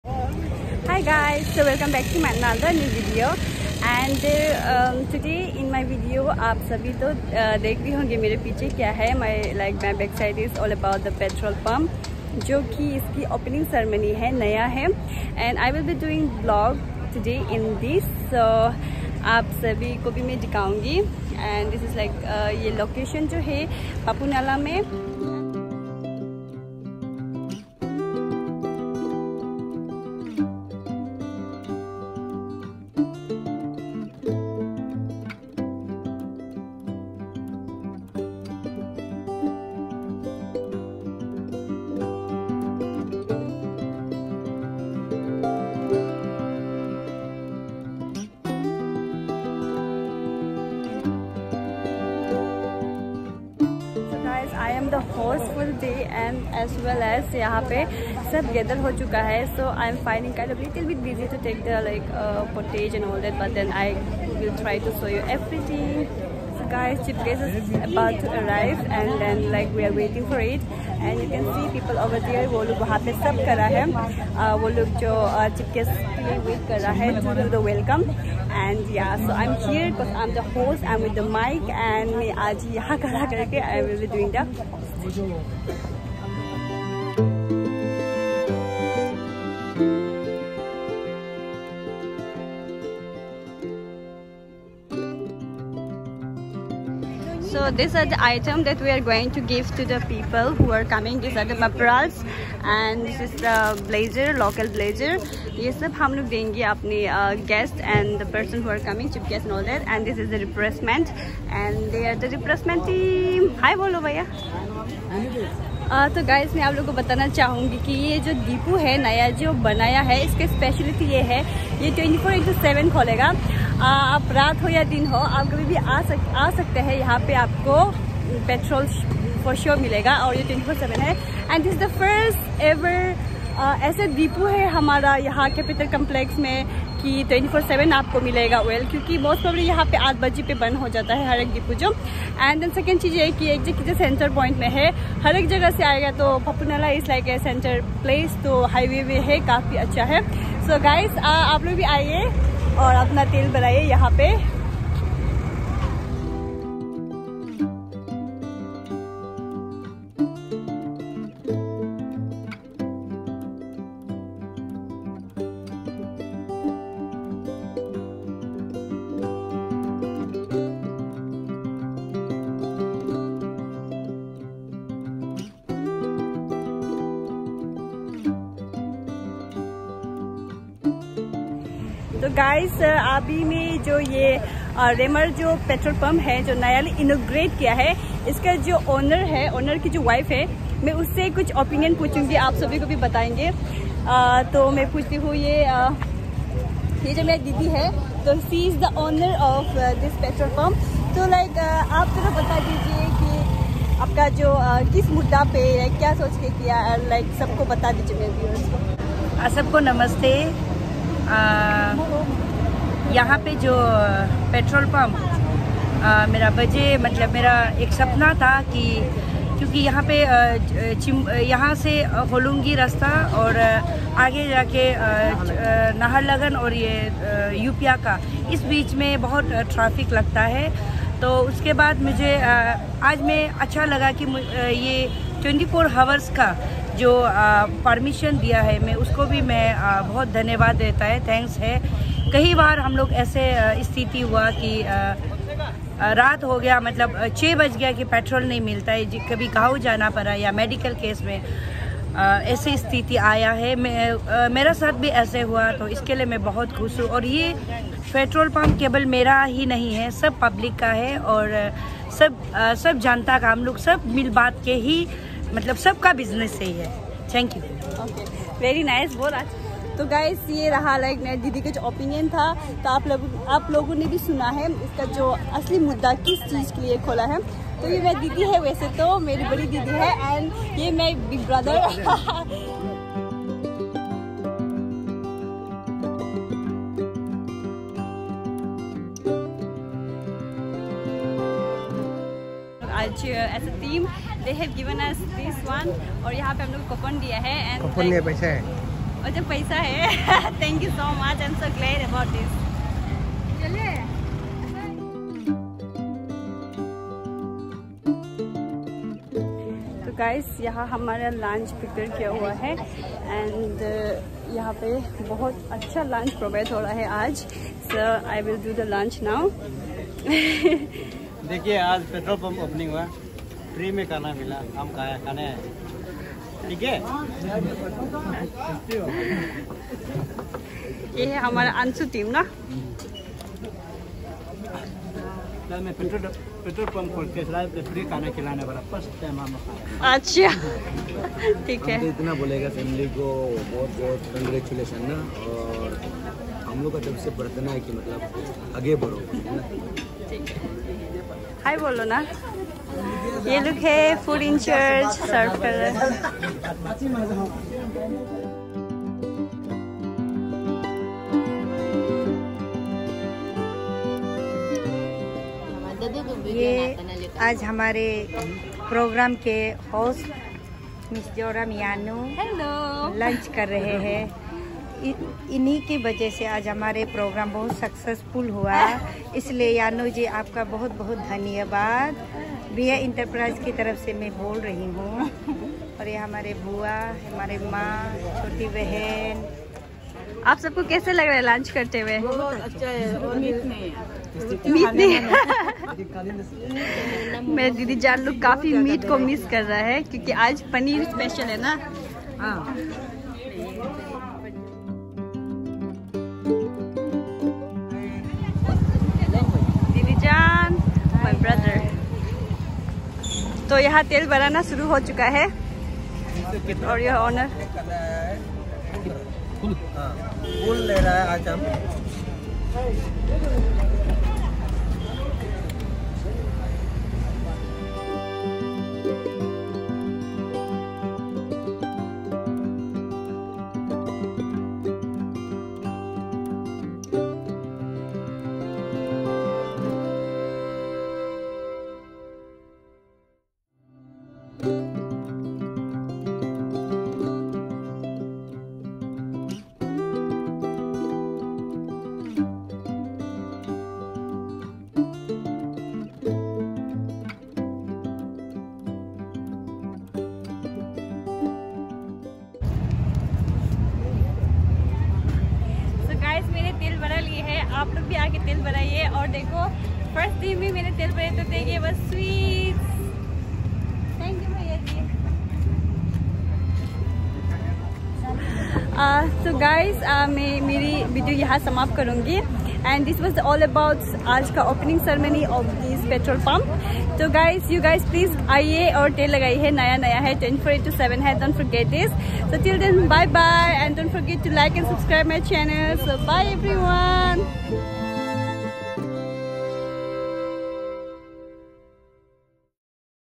Newly opened petrol pump in capital complex ll vlog || Joram yanu
Hi everyone !! I hope everyone is good great this day, this video is all about  newly opened petrol pump in papunalla  capital complex which is really amazing and at the same time petrol is available 24 hrs so you can go and get it anytime, it’s also  an  information for all my YouTube family , hope you guys enjoy this video ❤️